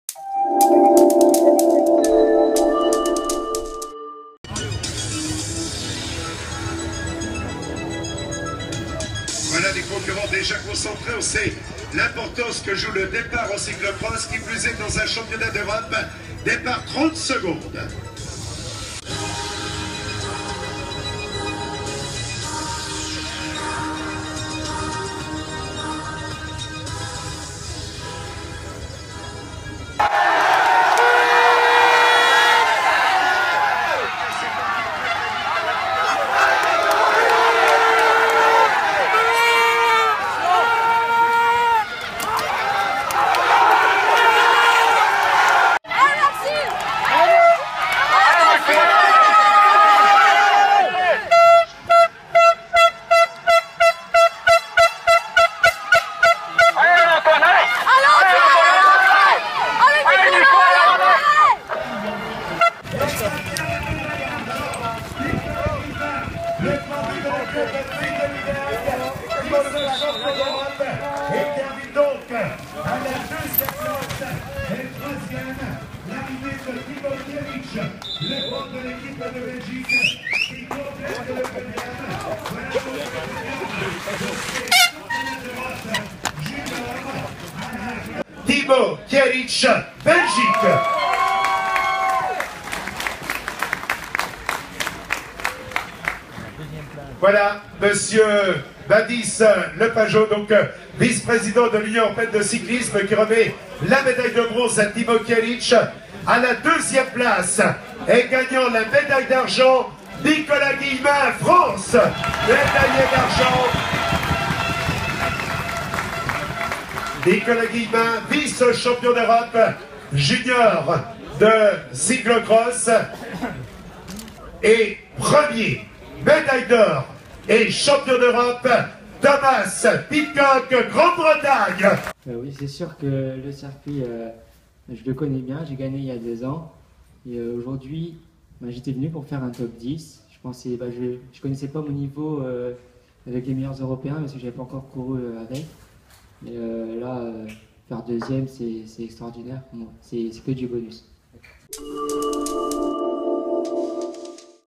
Championnat d'Europe de cyclo-cross. Pidcock vainqueur, Guillemin deuxième
Cyclisme ; Cyclo-cross ; Championnat d'Europe